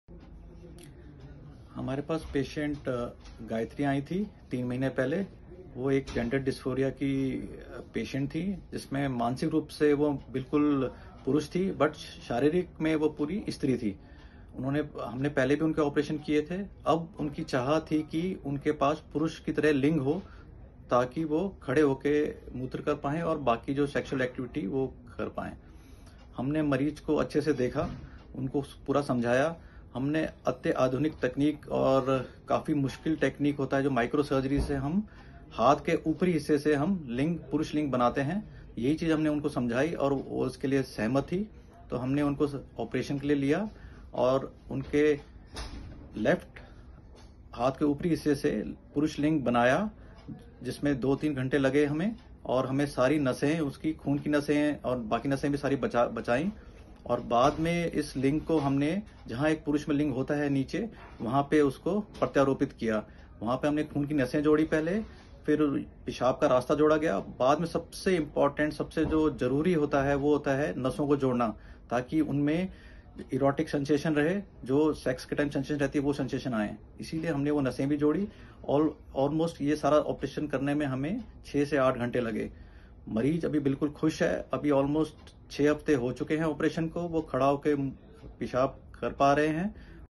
FEMALE TRANSFORMED INTO MALE BY PENILE RECONSTRUCTION FROM FOREARM
Department of Plastic & Cosmetic Surgery, Sir Ganga Ram Hospital, received a 34 years female Gayatri, (name changed) 2 months back from Uttar Pradesh who wanted to get completely transmitted into a male. By all proportions and psychiatric evaluations, it was found that although the patient was female mentally she was a male, a condition which is called Gender Dysphoria.

In the last 6 years, Gayatri had already undergone bilateral breast removal in 2017 and uterus, ovary, and vaginal removal in 2019. She was on male hormone replacement therapy since 2016. At the time of arrival at Sir Ganga Ram Hospital, the patient had all male characteristics including a beard, hairs on the chest, a male voice, and male behaviors. The only requirement was Penile reconstruction (Phalloplasty) and an implant to transform her into male.